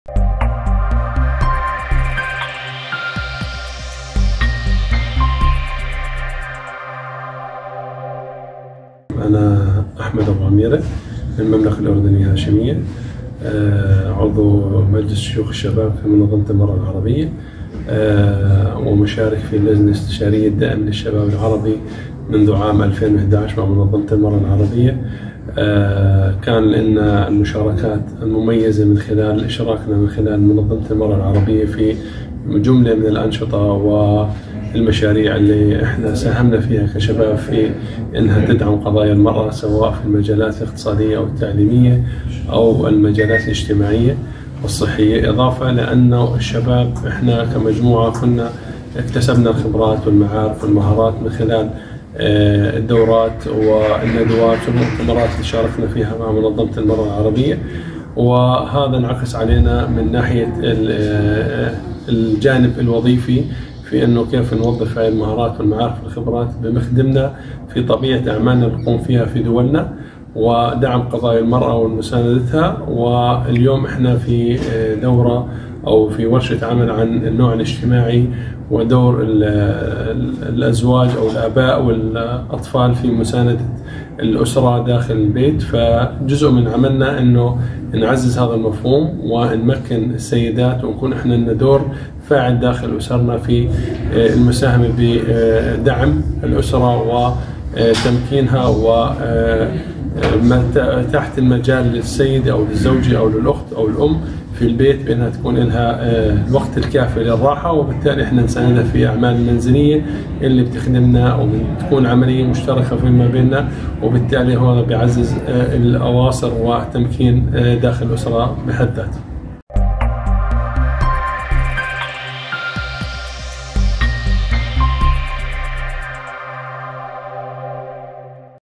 كلمة أ/ أحمد أبو عميرة، من المملكة الأردنية الهاشمية، على هامش ورشة عمل الشباب بسلطنة عمان
كلمة أ/ أحمد أبو عميرة، من المملكة الأردنية الهاشمية، على هامش ورشة العمل الإقليمية "دور الشباب بالمنطقة العربية في تعزيز مشاركة الرجال والفتيان في الأعمال المنزلية والرعائية" التي تعقدها منظمة المرأة العربية بالتعاون مع هيئة الأمم المتحدة للمرأة – المكتب الإقليمي للدول العربية على مدار يومي 5-6 يونيو/حزيران 2023.
(مسقط 5-6 يونيو 2023)